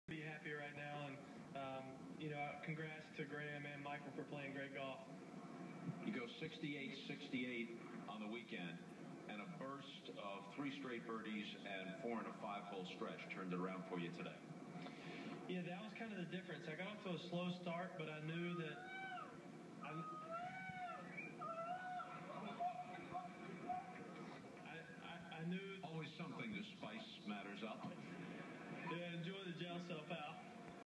British Man 2012 US Open Awards Ceremony
A British man makes a bird call during the 2012 US Open awards ceremony.